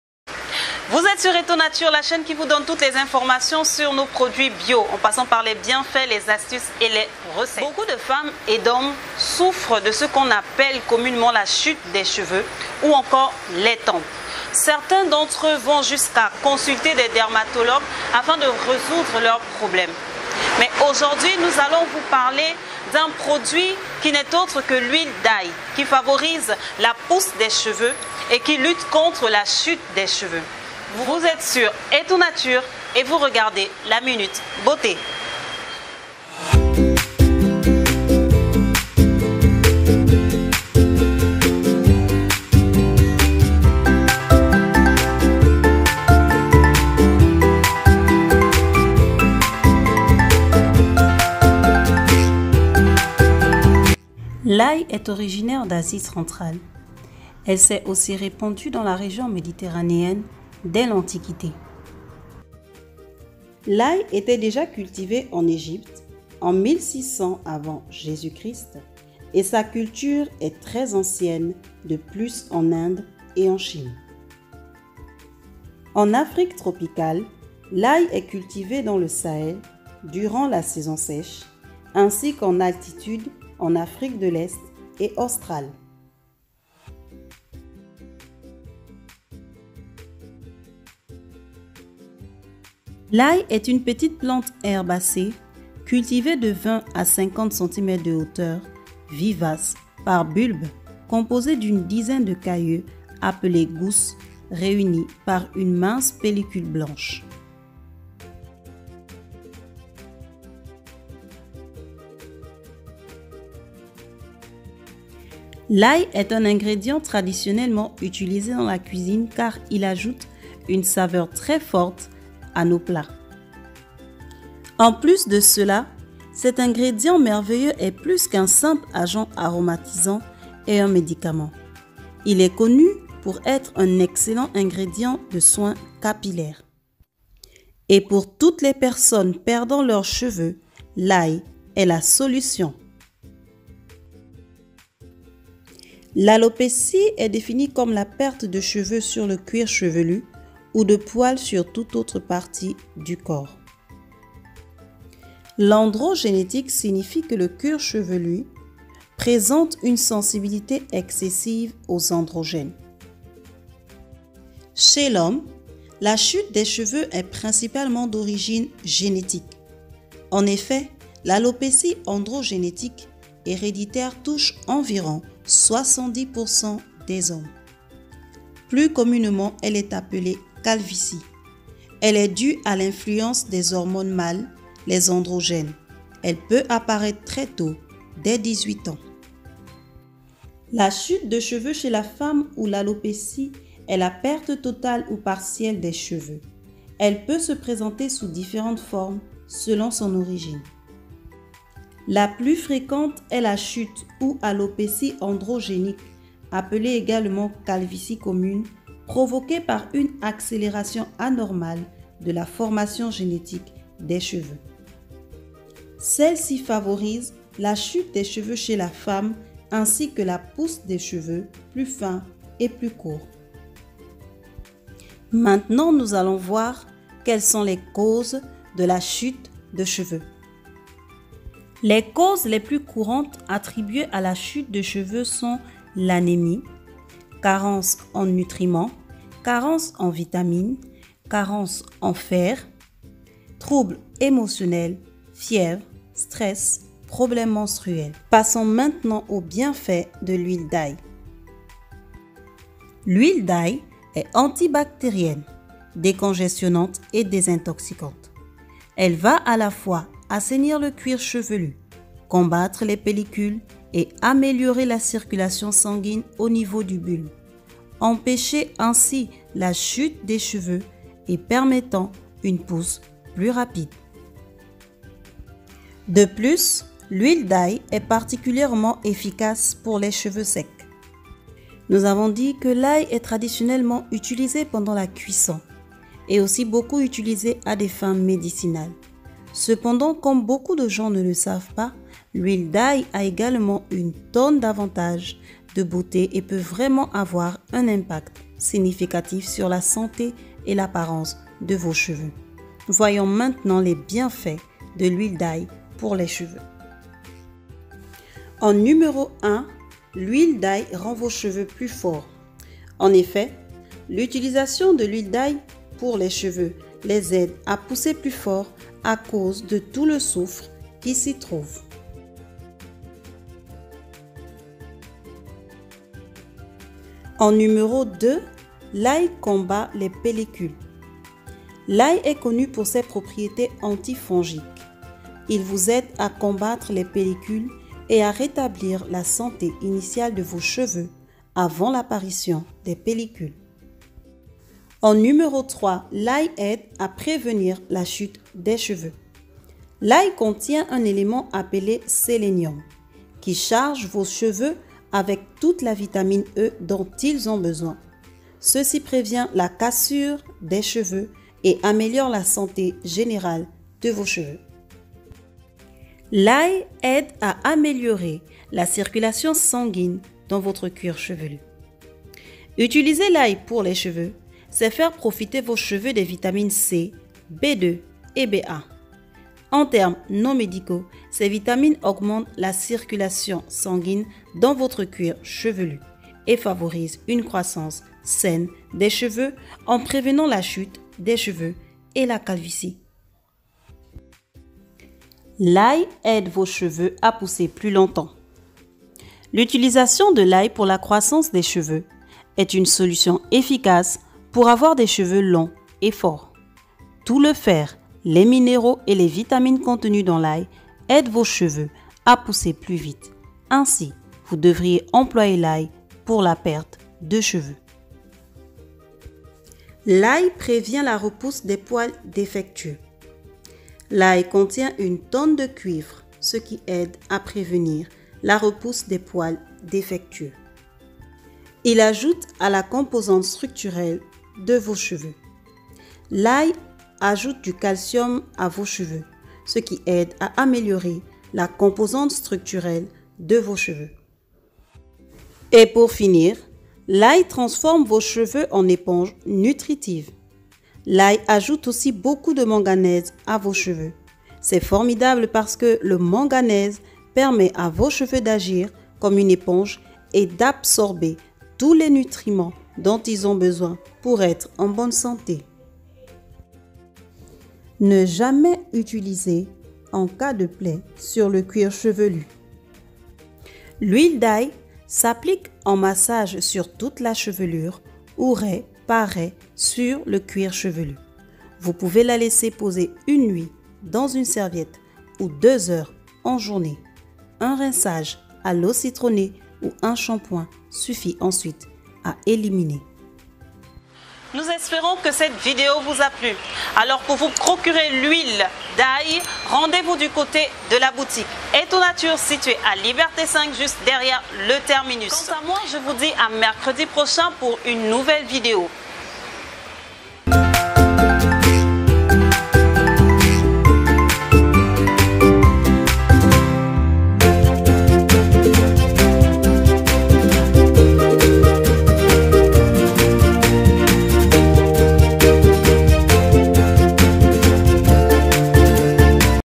Problèmes de tempes: Huile d'ail
L’ail est originaire d’Asie centrale. Elle s’est aussi répandue dans la région méditerranéenne dès l’antiquité. Il était déjà cultivé en Egypte en 1600 avant Jésus Christ, et sa culture est très ancienne, de plus en Inde et en Chine.  En Afrique tropicale, l’ail est cultivé dans le sahel durant la saison fraîche, ainsi qu’en altitude en Afrique de l’Est  et australe.

Description de la plante : petite plante herbacée, cultivée, de 20 à 50 cm de hauteur, vivace par bulbe composé d’une dizaine de caïeux appelés gousses réunis par une mince pellicule blanche.

A quoi sert l’ail?
 L'ail est un ingrédient traditionnellement utilisé dans la cuisine car il ajoute une saveur très forte au plat. En plus de cela, cet ingrédient merveilleux est plus qu'un simple agent aromatisant et un médicament ; il est connu pour être un excellent ingrédient de soins capillaires et pour toutes les personnes perdant leurs cheveux, l'ail est la solution.

Quelques définitions:
Alopécie: Perte de cheveux sur le cuir chevelu ou de poils sur toute autre partie du corps.
Androgénétique: En premier lieu, il faut savoir que nos cheveux se développent et vivent sous dépendance hormonale.
Une prédisposition “androgénétique“ signifie que votre cuir chevelu présente une sensibilité excessive aux androgènes. Hormones mâles, sécrétées aussi par les femmes en très petite quantité, les androgènes circulent normalement dans le sang.

Pour tous nos produits naturels merci de visiter notre site au ou de nous contacter au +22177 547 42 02 (Sénégal)